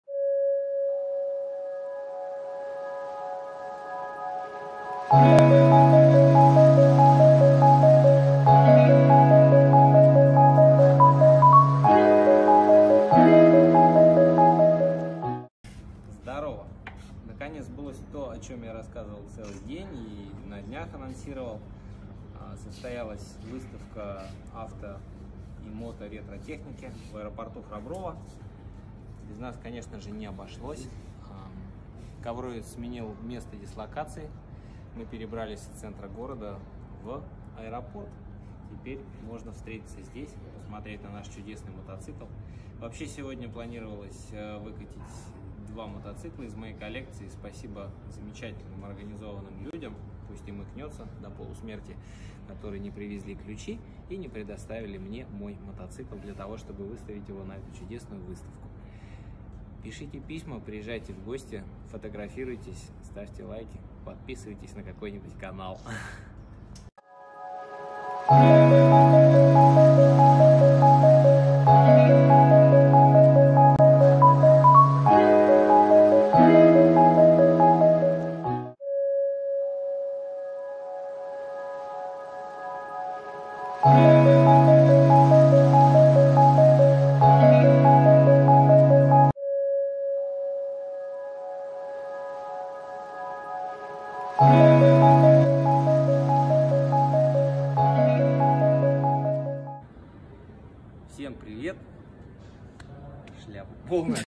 Ретро выставка в аэропорту Храброво.
СОБЫТИЕ невероятного масштаба для наших краёв! Авторетро клуб при поддержке Аэропорт Храброво организовал выставку техники в центральном зале аэровокзала! Теперь у всех есть отличная возможность получить эстетический оргазм перед вылетом)) P.S не забывайте, те кто лапает руками чужую технику рано или поздно останутся без пальцев!!! Наряду с уже наблюдающимся отсутствием мозгов это будет крайне прискорбно! аэропортхраброво✈️